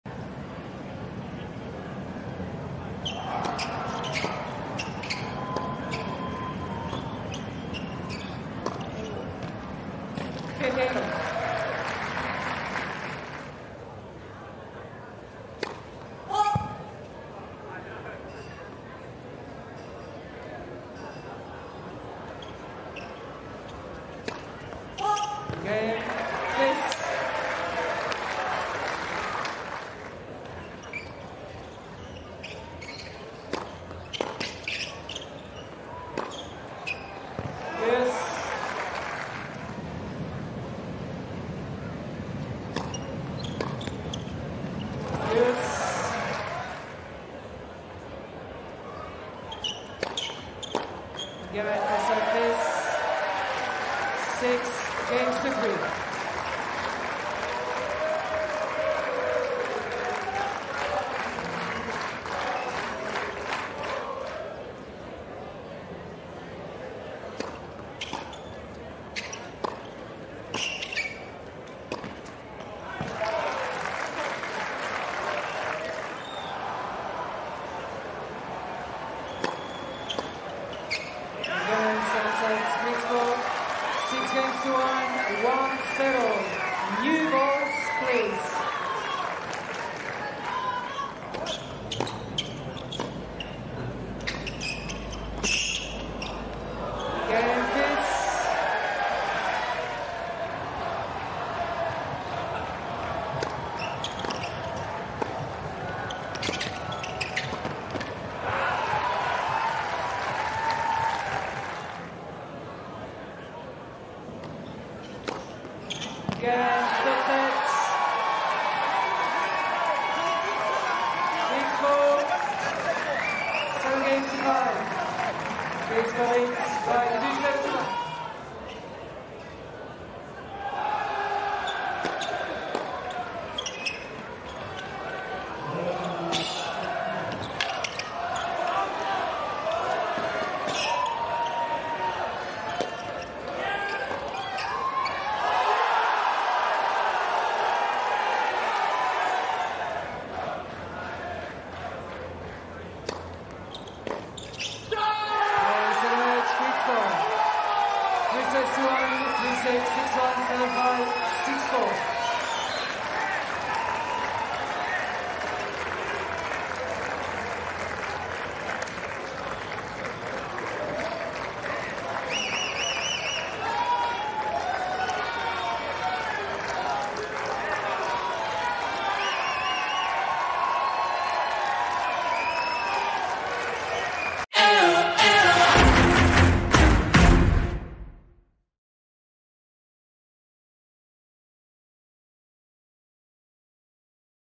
Arthur Fils v Tallon Griekspoor Highlights | Australian Open 2024 Second Round
Watch the highlights of Arthur Fils v Tallon Griekspoor in the second round of the Australian Open 2024.